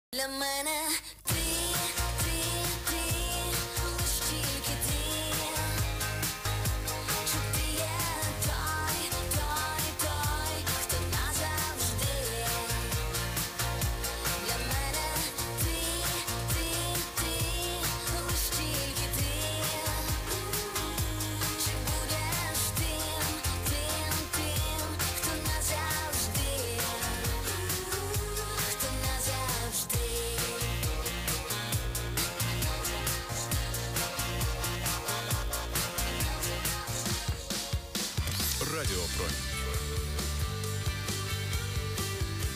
Ukrainian broadcasting VHF OIRT - Radio Promin (at a distance of about 100 km).
Ukrainian broadcasting VHF OIRT - Radio Promin (about 100 km to the Tovsta Gora transmitter, Khust, 500 W) at a frequency of 71.9 MHz,YouLoop Airspy antenna on the balcony window (my other antennas do not accept this),RSPdx,SDRuno.Ukraine,QTH KN18DP.